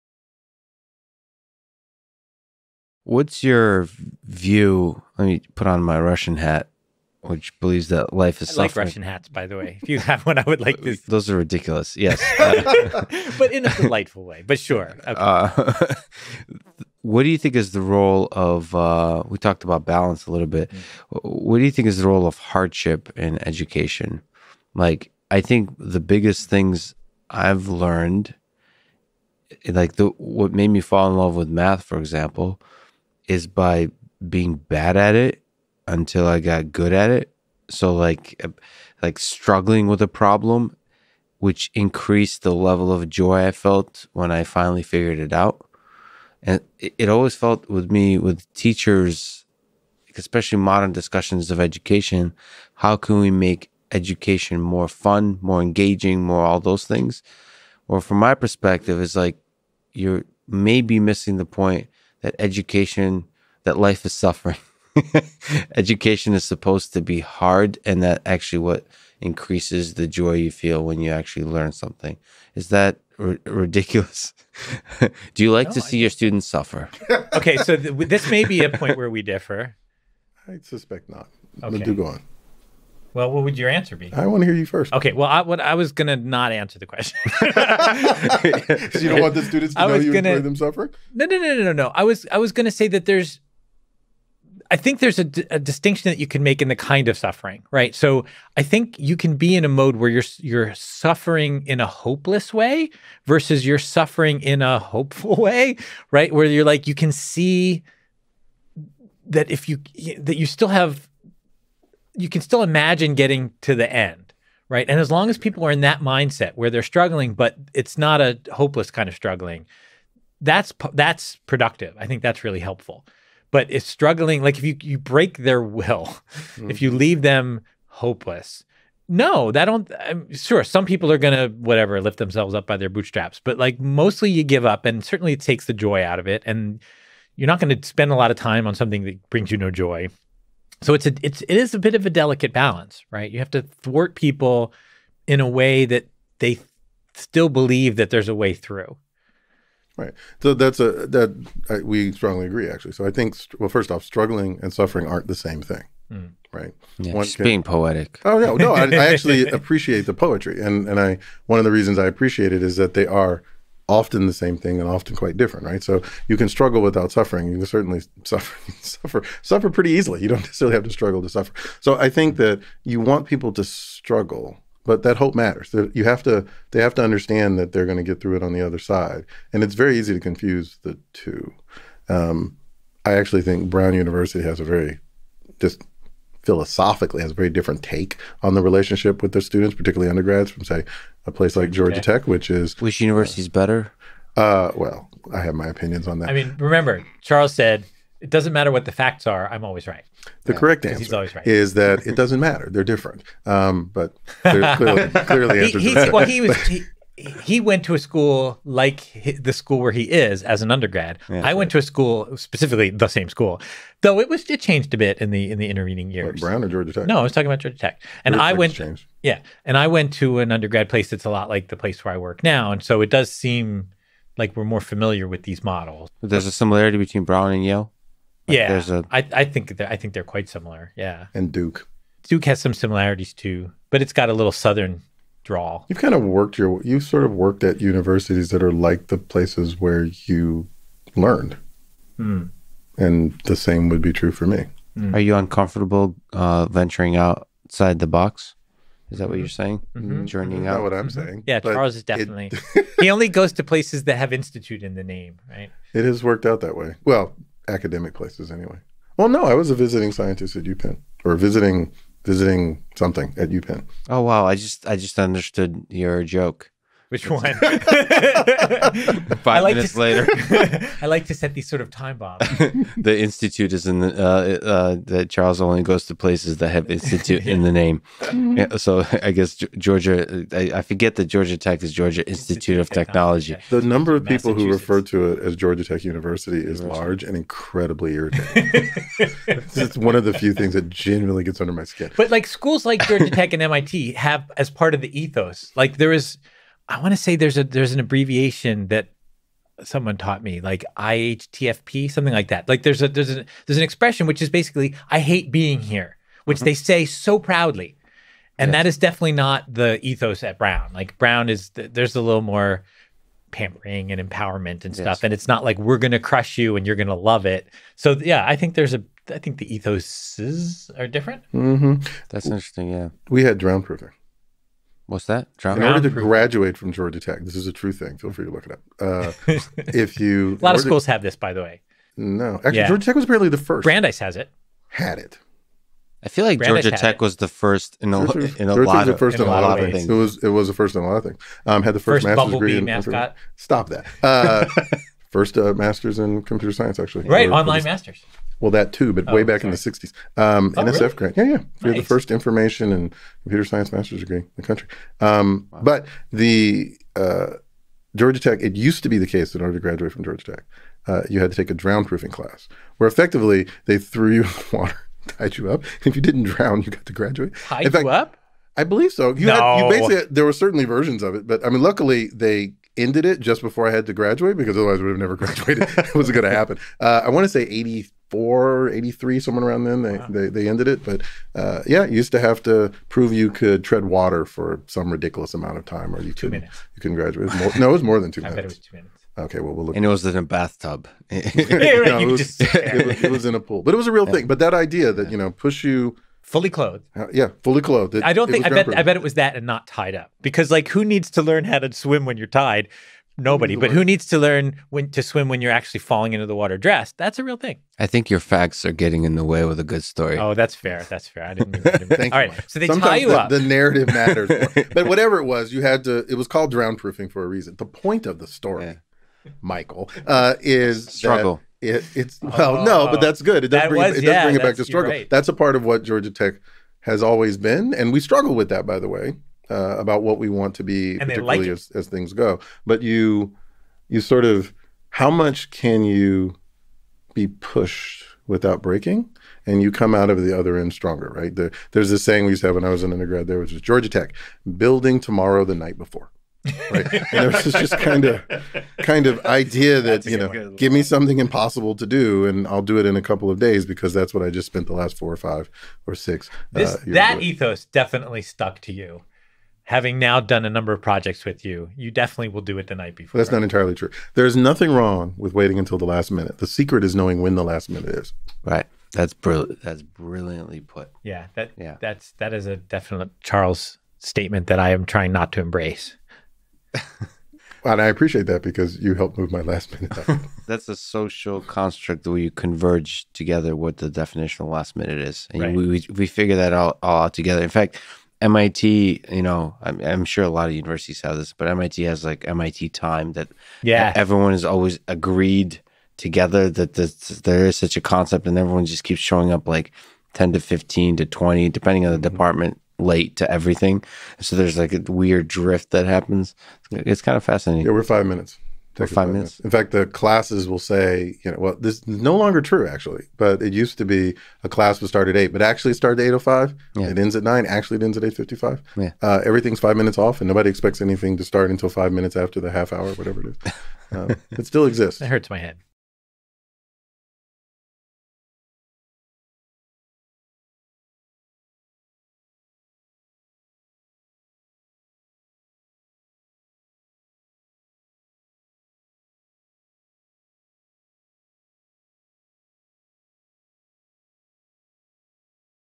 Role of struggle and suffering in college | Charles Isbell and Michael Littman and Lex Fridman
- MasterClass to get 2 for price of 1